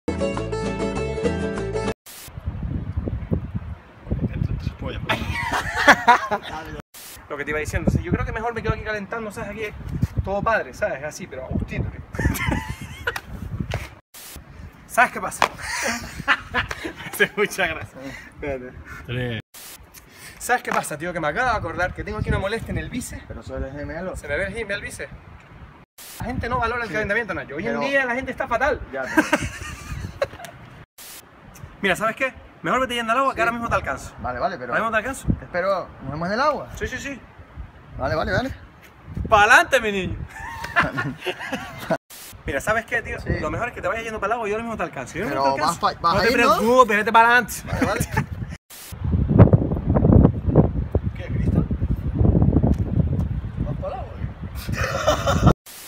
SPOT QUEMAO CLASS 2016 - Tomas Falsas
No están todas las que fueron pero sí son todas las que están. Algunas risas que nos echamos grabando el spot para Quemao Class​ estas navidades :)